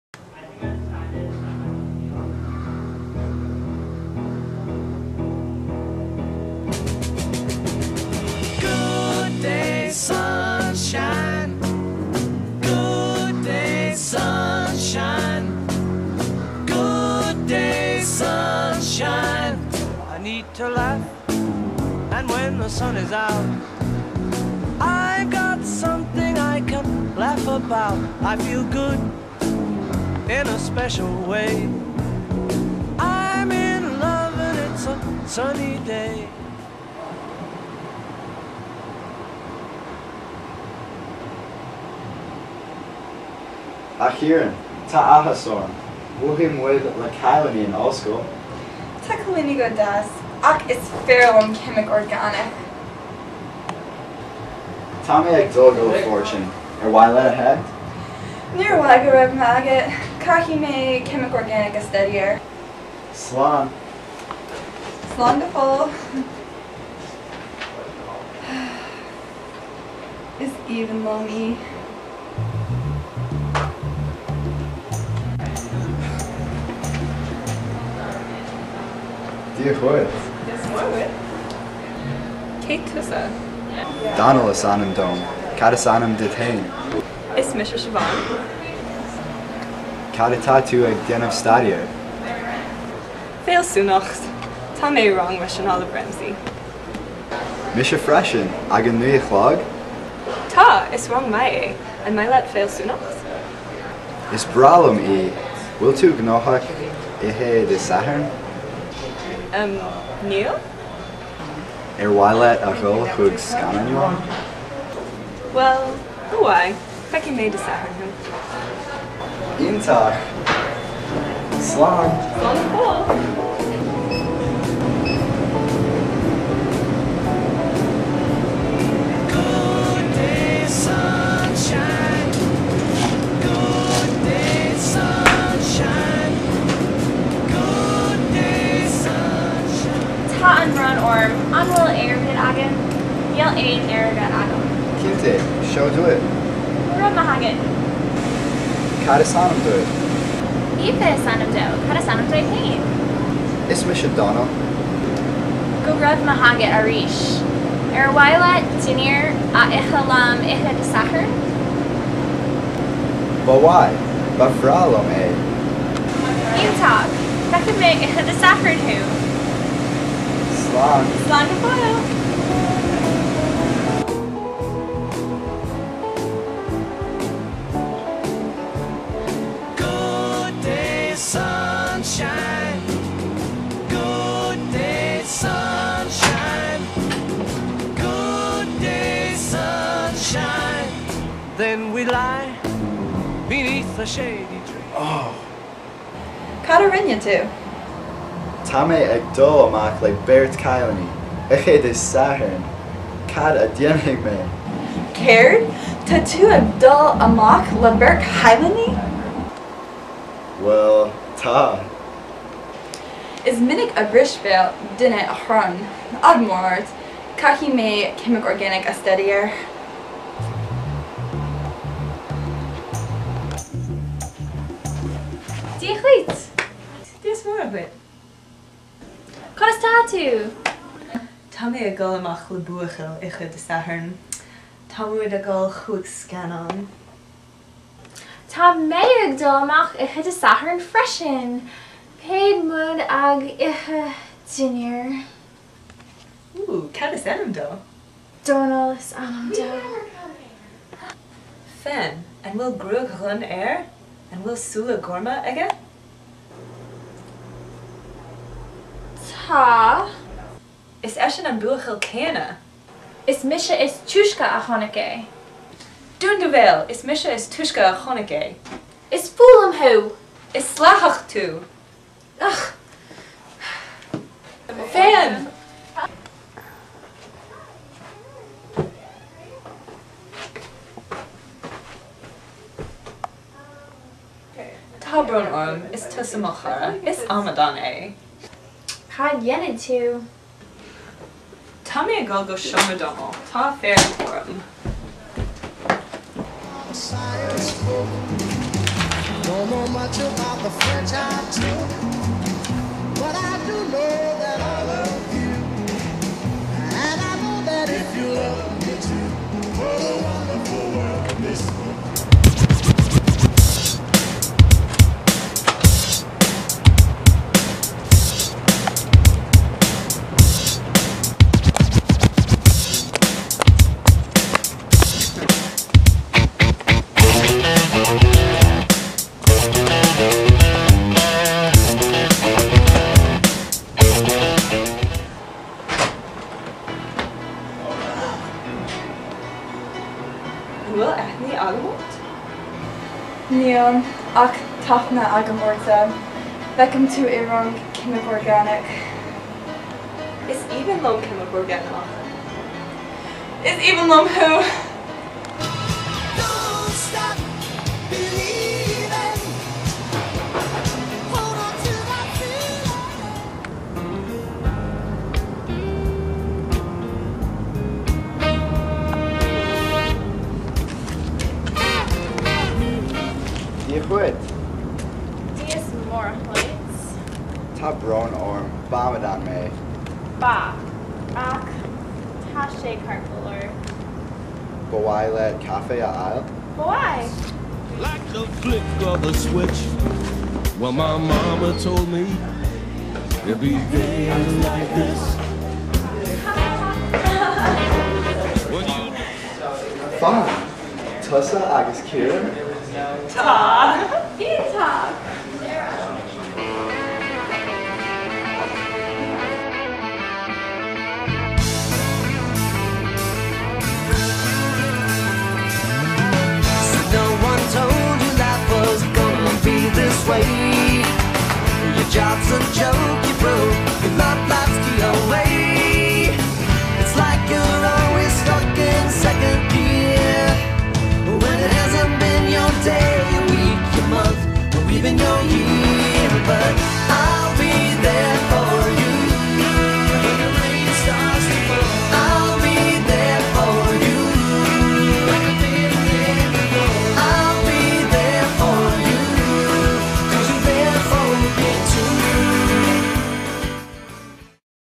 Grá, Gráin agus Ceimic Orgánach (Love, Hate and O-Chem)
Miondráma as Gaeilge déanta ag mic léinn Notre Dame sa tríú téarma (an Irish-language skit/melodrama, "Love, Hate and Organic Chemistry," made by Notre Dame students in their third semester of the language)